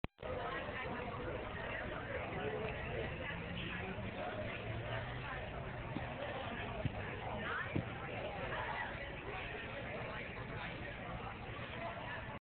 Roundoff, Back Hand Spring, Backflip Gymnastics
I tried out a backflip for my first time! It is prefaced with a roundoff, and back hand spring. It's with spotter, just in case. Doing some gymnastics.